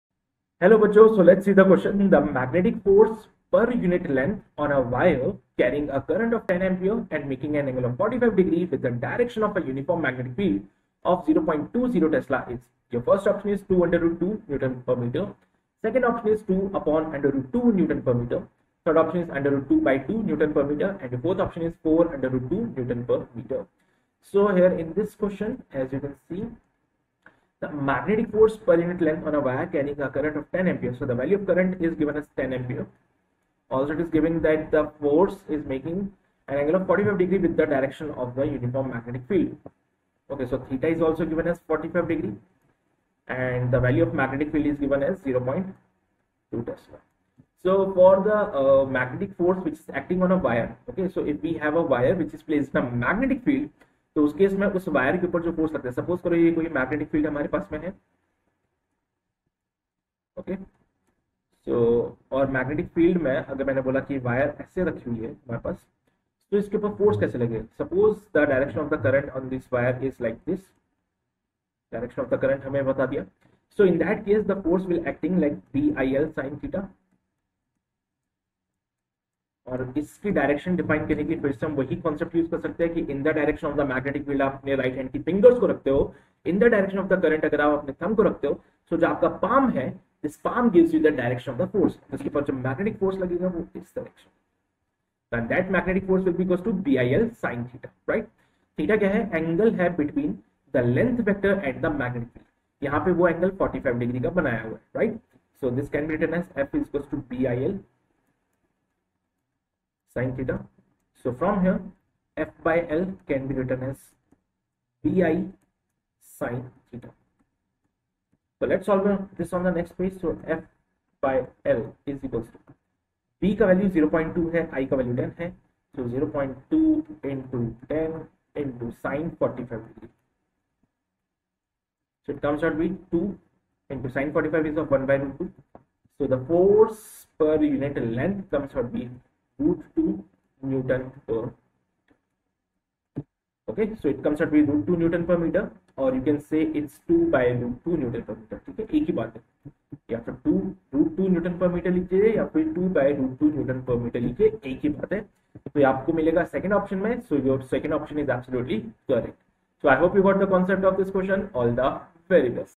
The magnetic force per unit length on a wire carrying a current of 10 A and making an angle of 4...
The magnetic force per unit length on a wire carrying a current of 10  A and making an angle of 45^∘ with the direction of a uniform magnetic field of 0.20  T is
a. 2 √(2)Nm^-1
b. 2/√(2)Nm^-1
c. √(2)/2Nm^-1
d. 4 √(2)Nm^-1